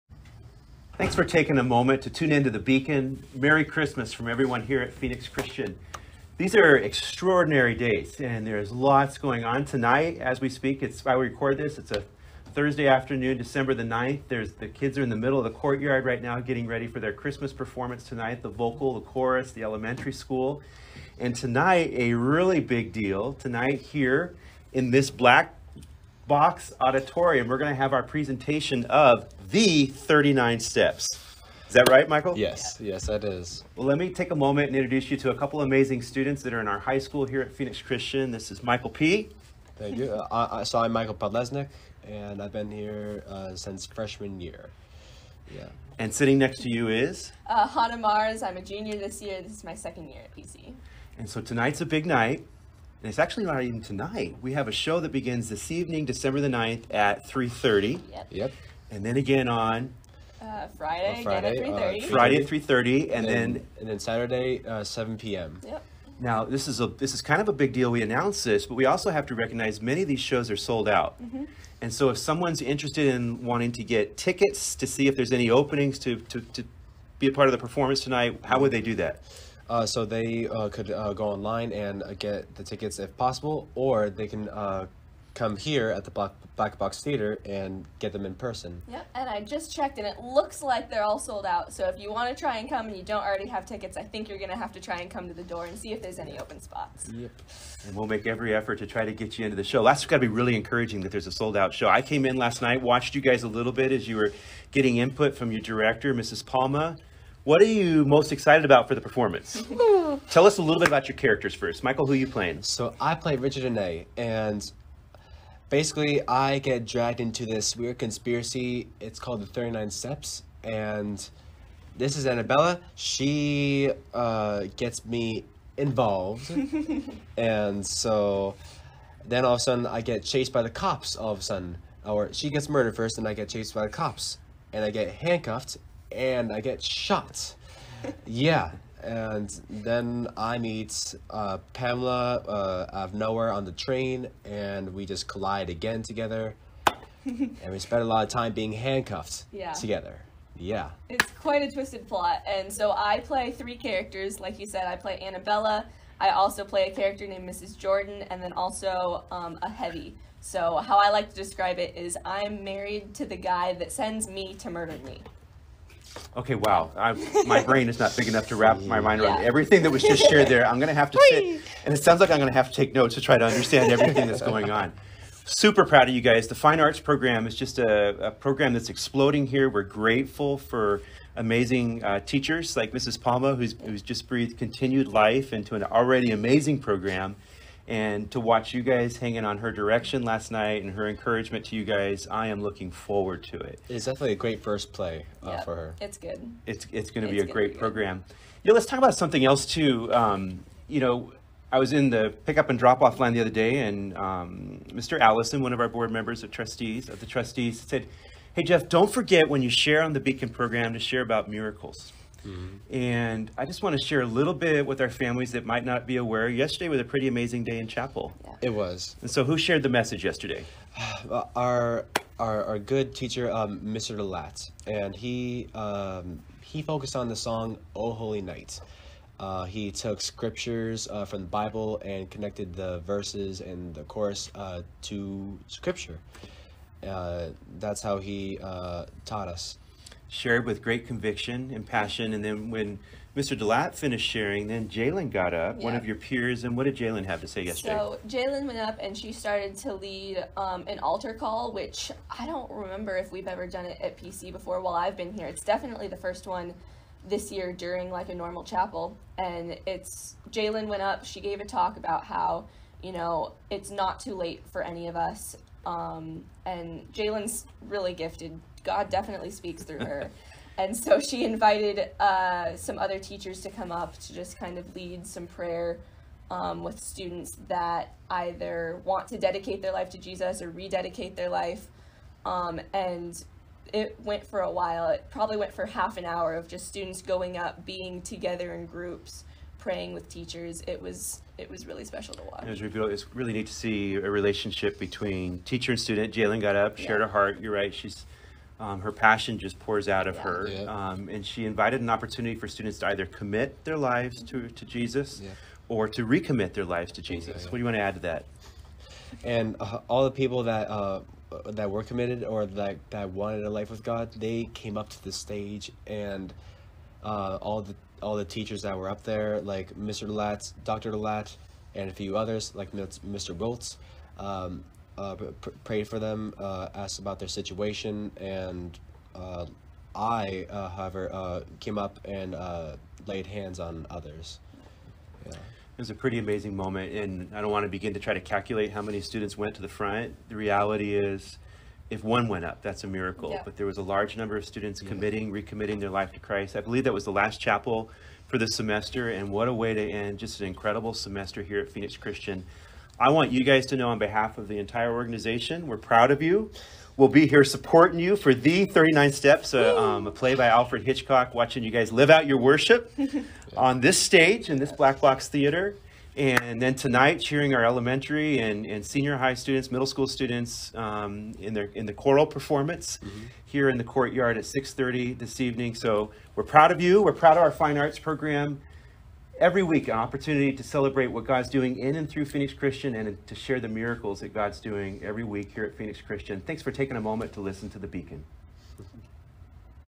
39 Steps...
Hear about our amazing drama program, led by Elise Palma, through the lens of Michael and Hana, as they give us a sneak peak into the fall play, called 39 Steps by Alfred Hitchhock.

Show Dates: 12/9/2021-12/10/2021